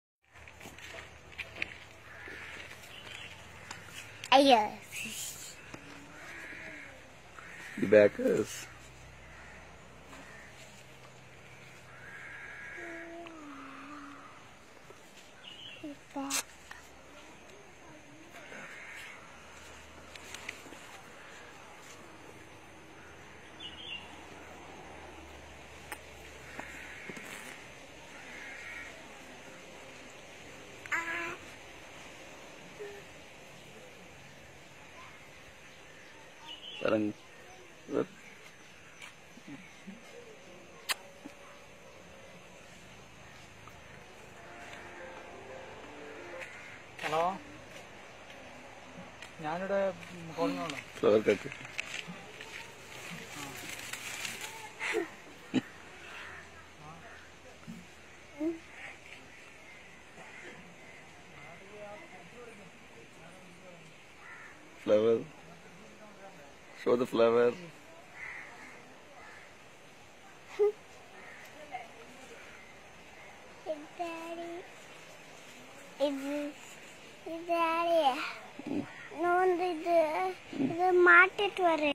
Padma Priya Babu ❤️❤️❤️ 9/6/2021 Karuvanchal Kannur Kerala daughter❤️
Padma Priya Babu ❤️❤️❤️ 9/6/2021 Karuvanchal Kannur Kerala

My Little Princess Padma Priya Babu ❤️❤️❤️
@ My In-laws Home Karuvanchal Kannur Kerala India 🇮🇳
on 09/06/2021 ...
Memories Video Clip